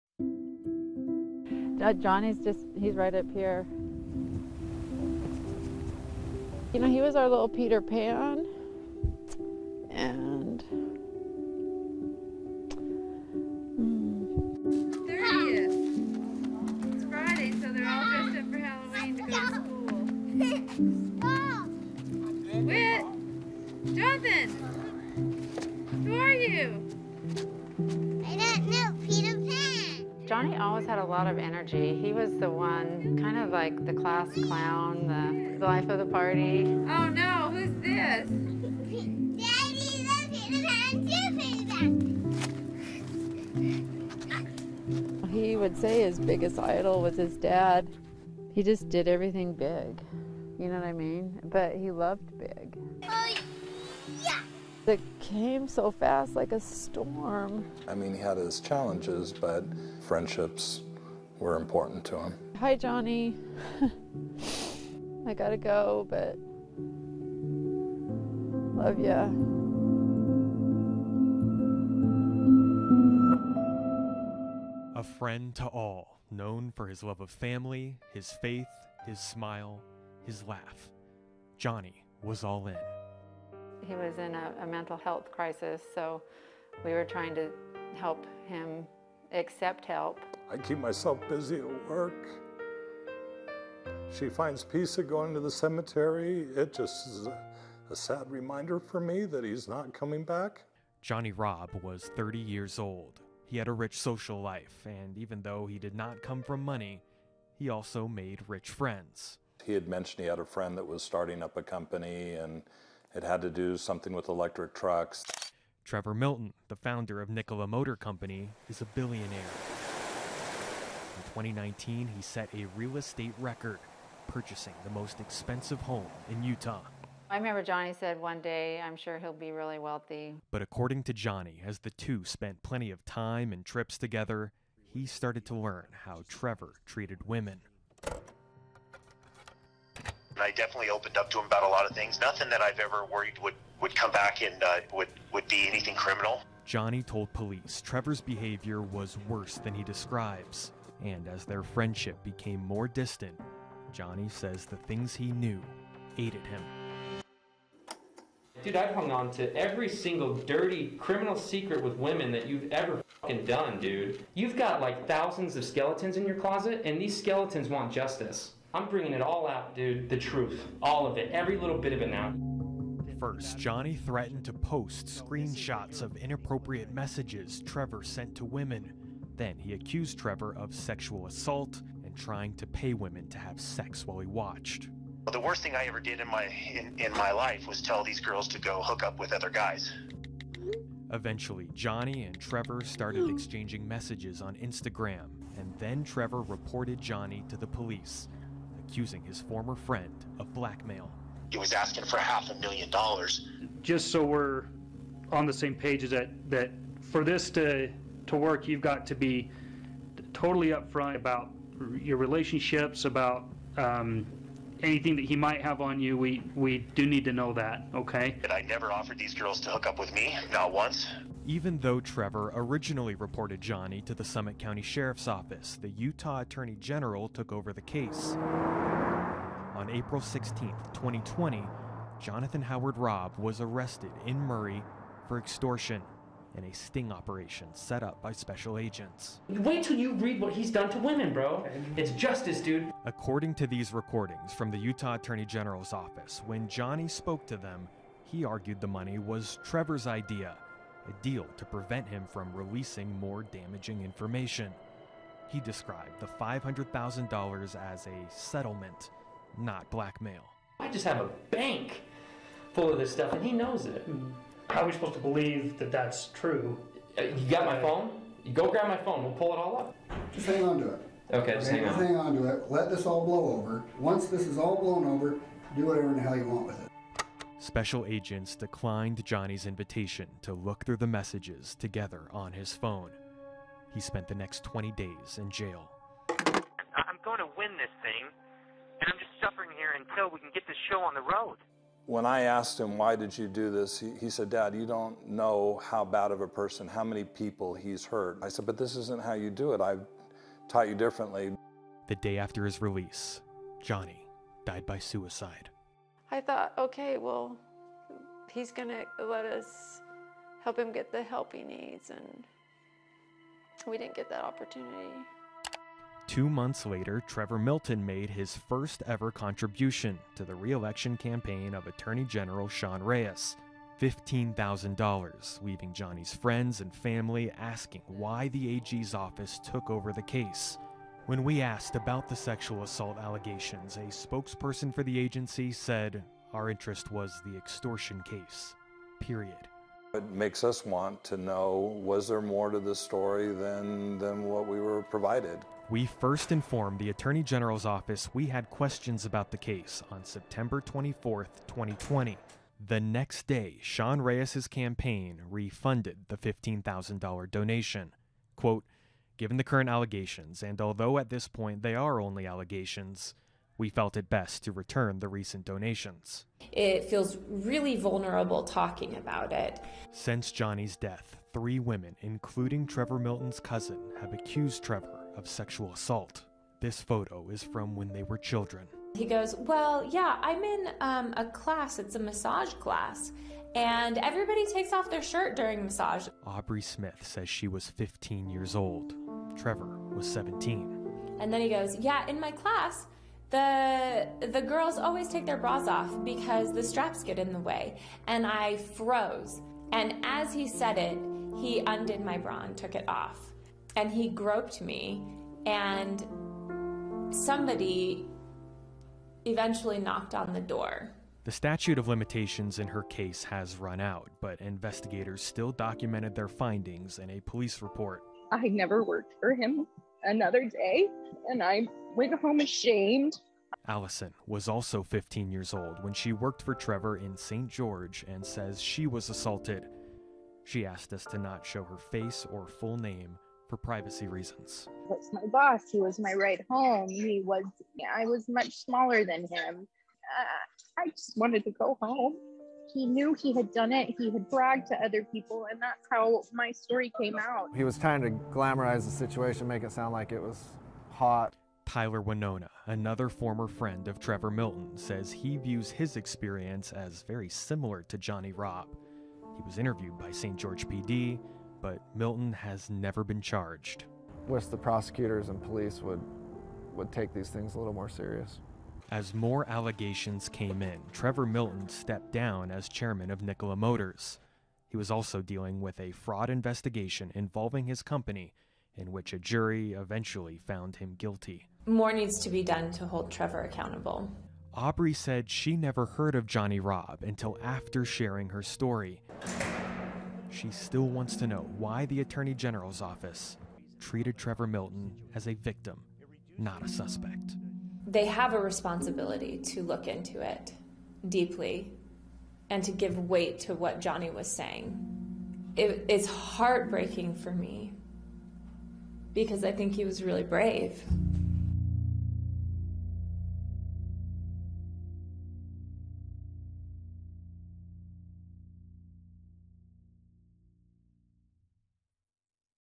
Billionaire and future campaign donor not investigated for sex assault
FOX 13 Investigates: Jonny and Trevor